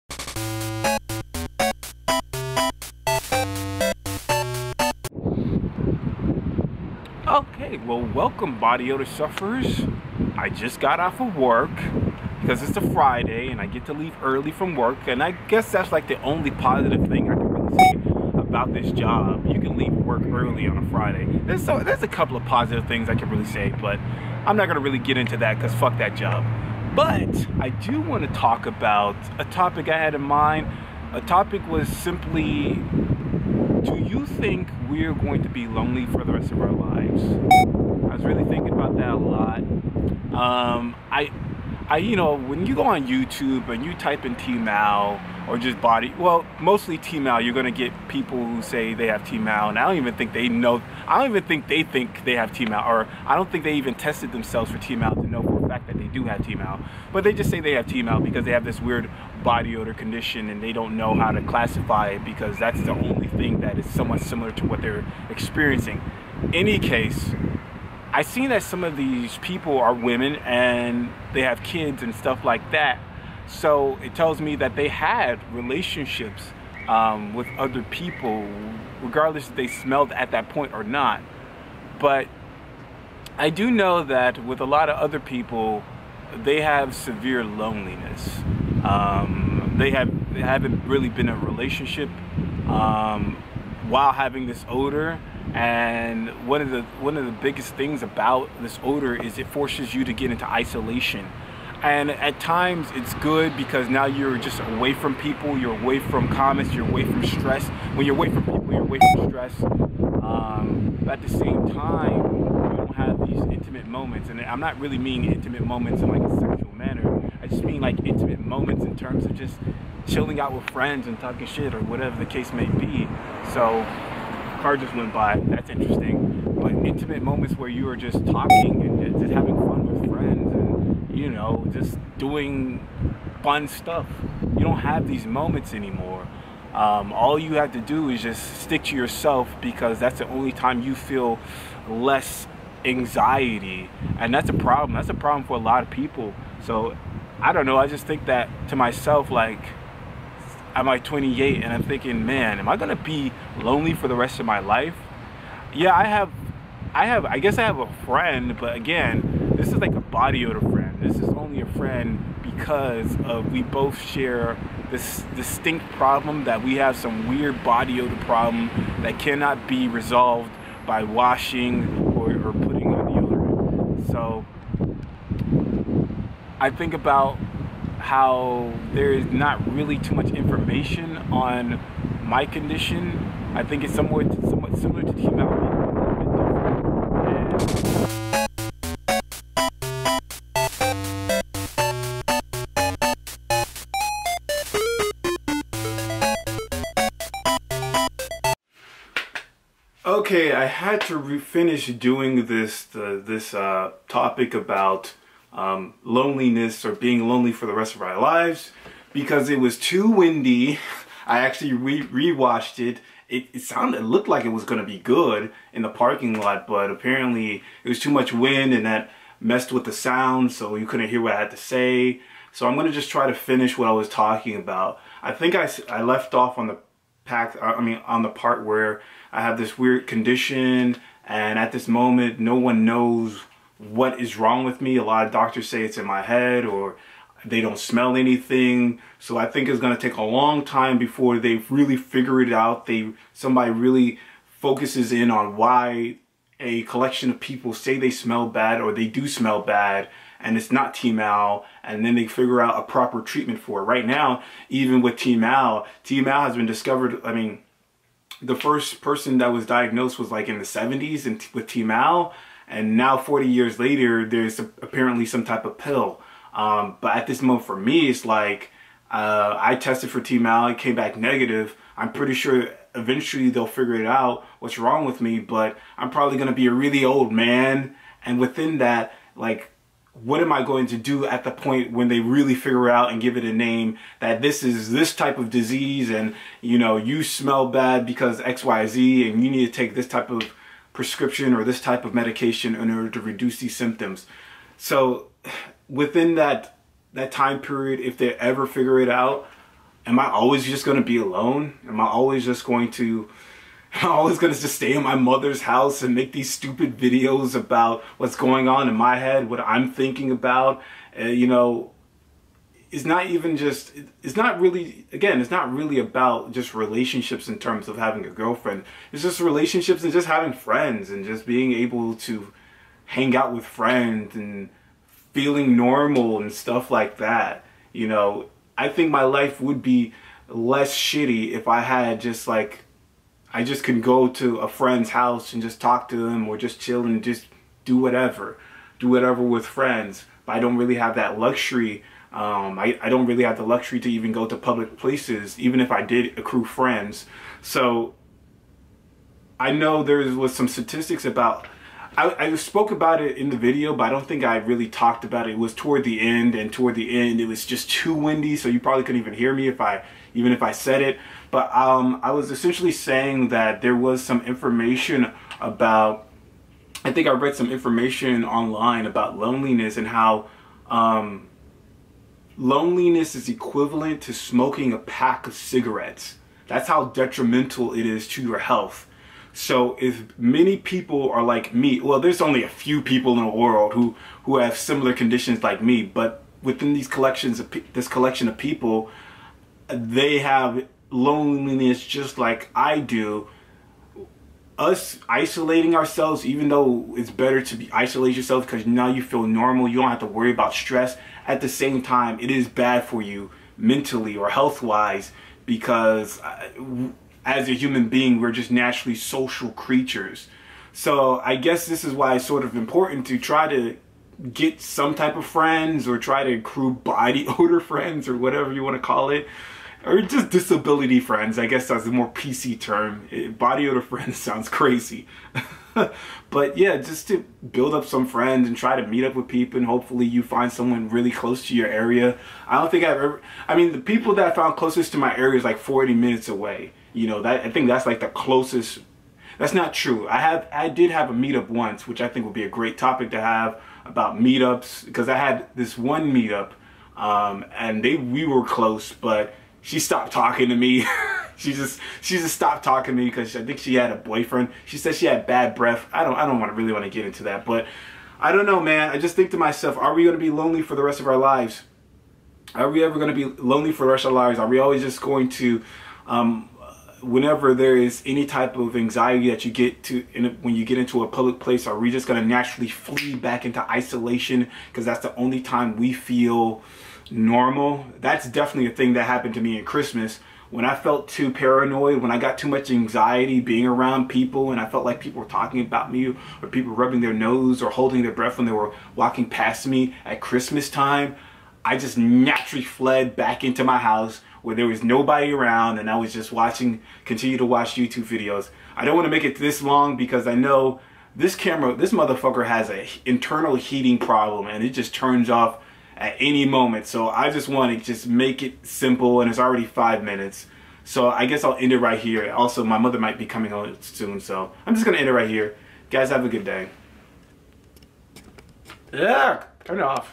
Topic: Are we going to be lonely for the rest of our lives
Have you ever had the feeling that you are going to be lonely forever with this condition? I always think about that, and it makes me sad and I just wanted to know how everyone else felt about their condition and if it i makes them feel lonely